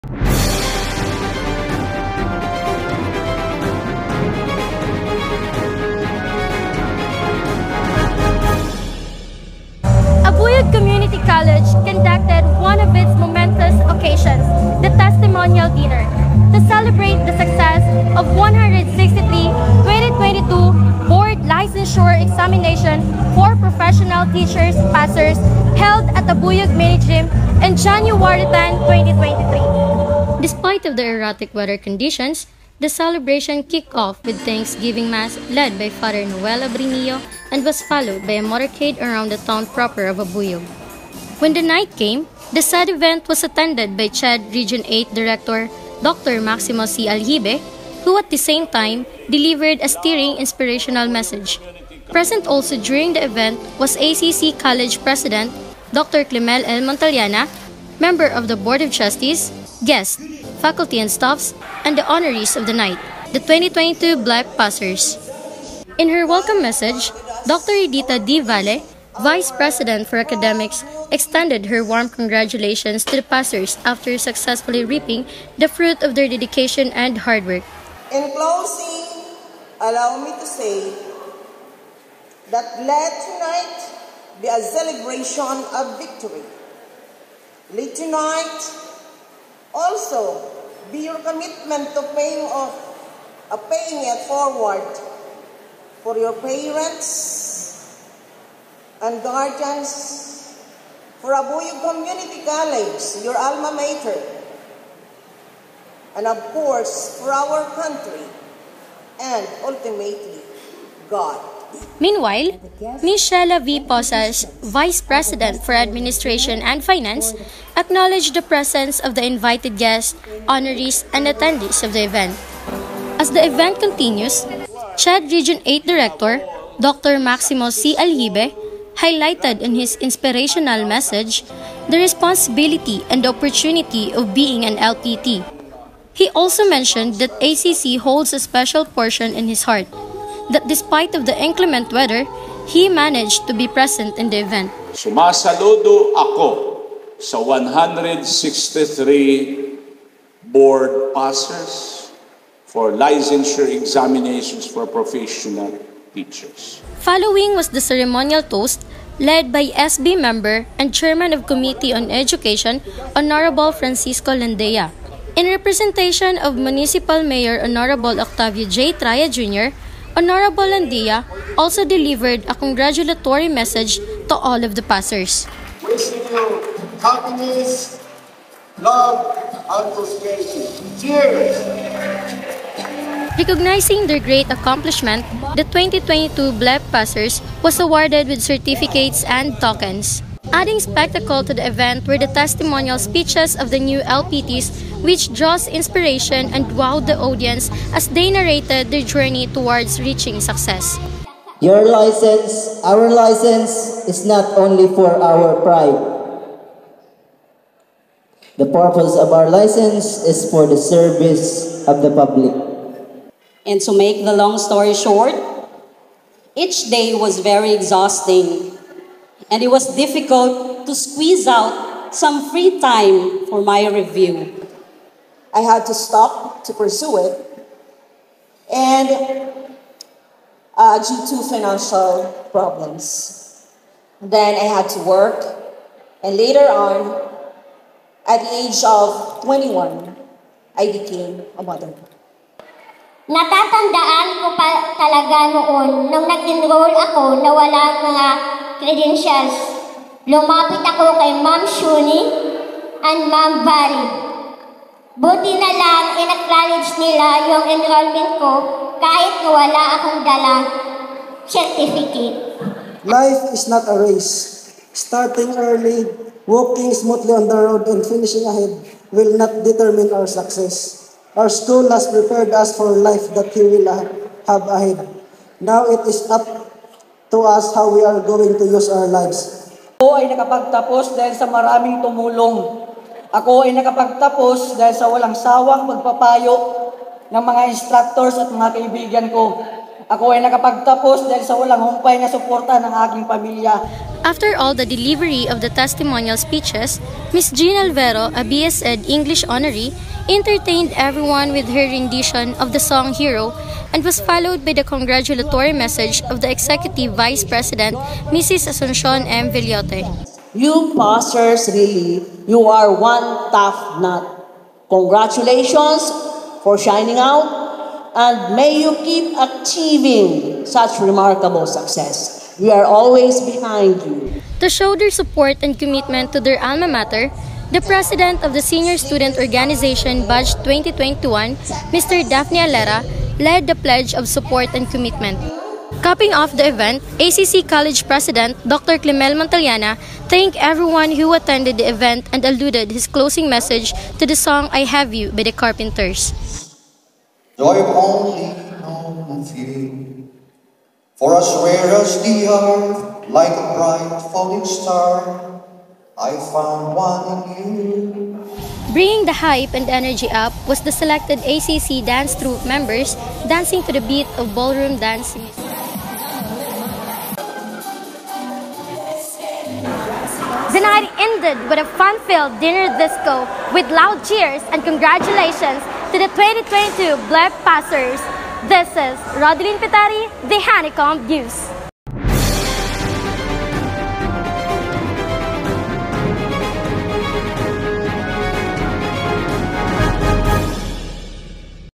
ICYMI. ACC Testimonial Dinner: A recognition of the great success of its newly- produced LPT's
Beavering its commitment to be one of the renowned local community college in the region, it is with great pride that Abuyog Community College celebrates the success of the 2022 Board Licensure Examination for Professional Teachers (BLEPT) passers through its testimonial dinner held at Abuyog Mini Gym on January 10, 2023.

Here is Rodelyn Petere for the report.

Produced by| E. Declaro

For more updates like and follow The Honeycomb Campus Media